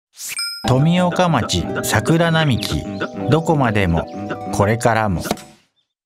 【富岡町】もっと 知って ふくしま！第2弾 夜ノ森桜並木【6秒】
国道6号線から夜の森公園に続く道に、樹齢100年のソメイヨシノが並び、見事なサクラのトンネルをつくっています。震災後、帰還困難区域に指定されていましたが、平成30年に一部制限が解除され、富岡町桜祭りも復活しました。